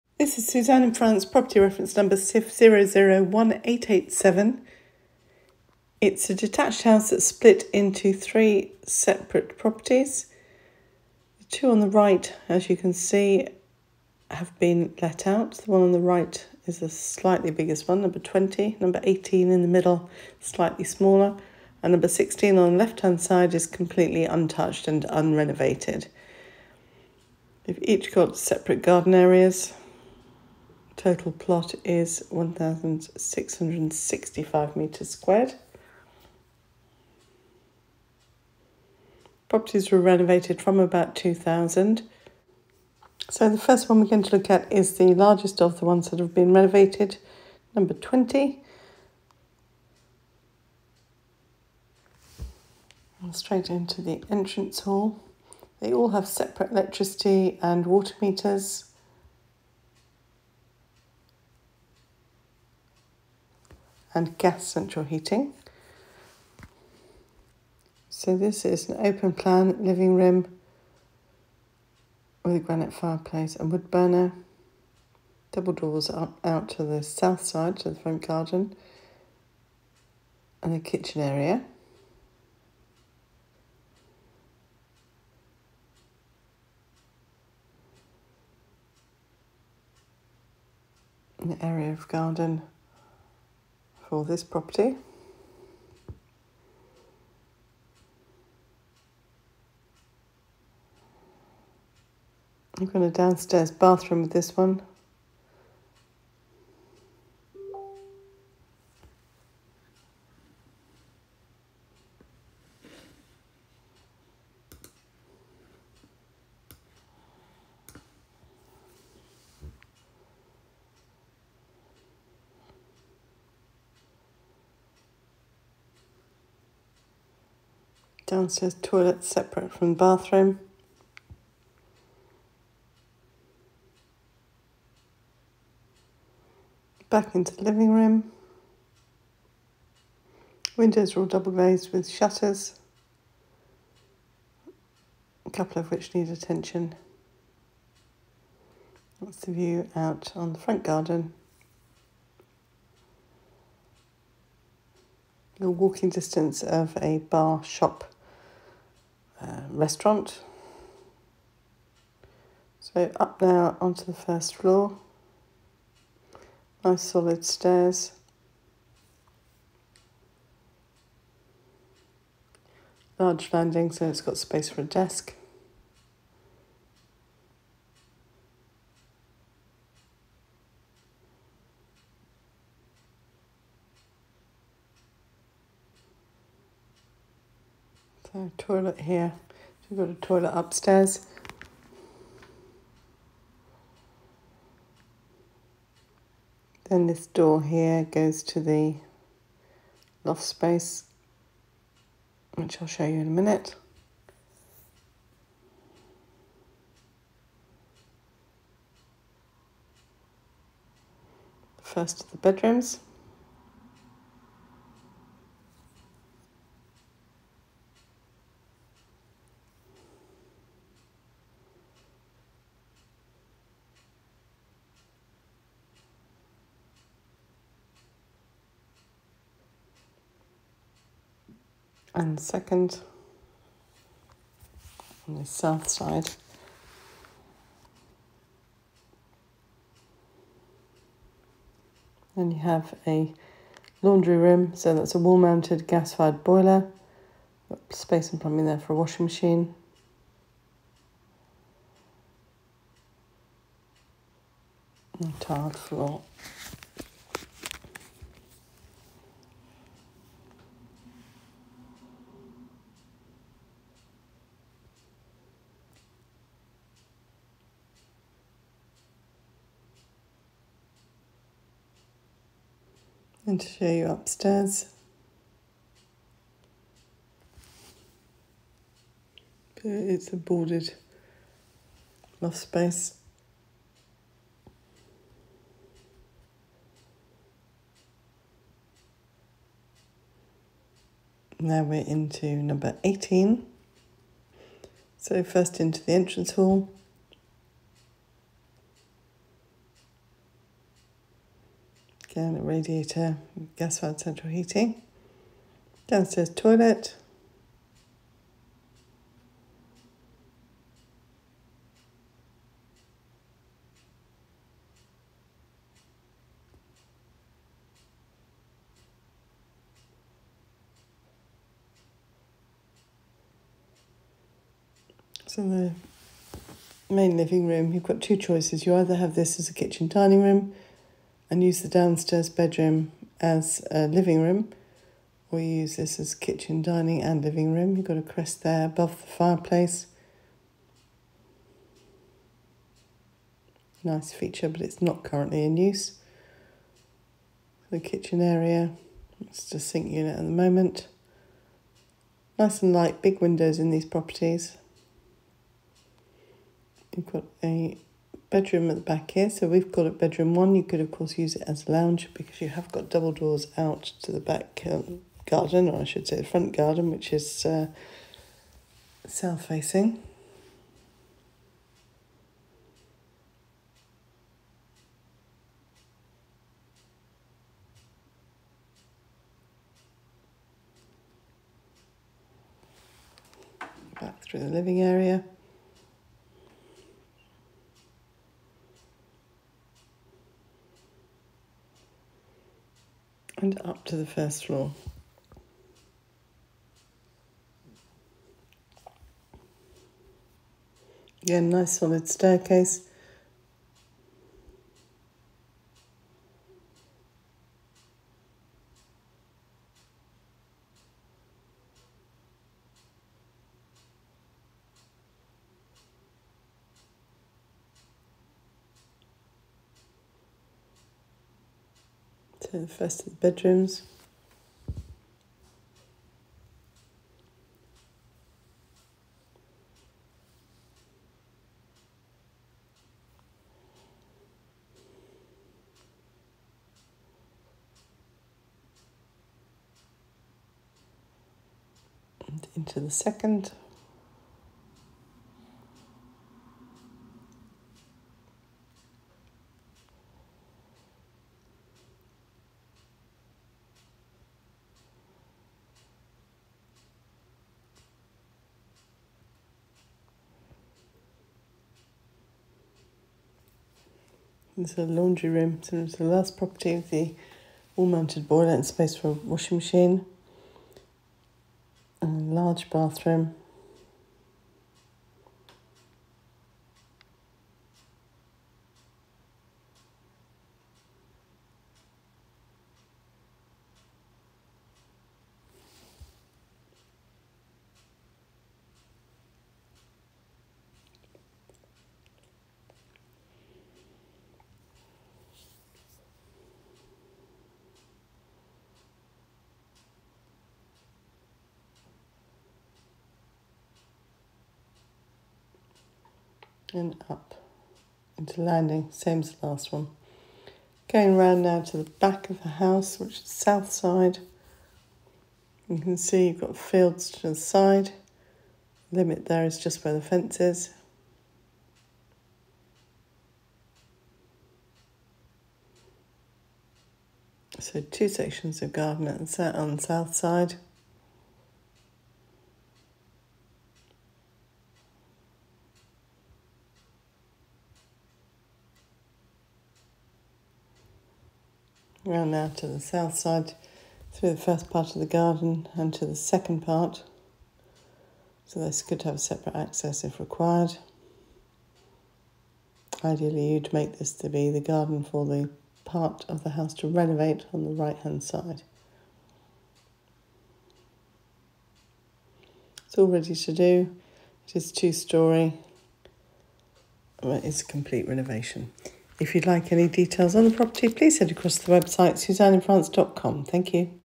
SIF -001887 - 3 attached village properties & 1 to renovate each with own garden
@SuzanneinFrance is delighted to show you this detached house divided into 3 attached village properties with 1 still to renovate each with their own garden

Asking price : 118,500€ including Agency fees of 8,500€.  In addition the buyer will pay the Notaire's fee - 9,500€

If you would like to search our other properties for sale please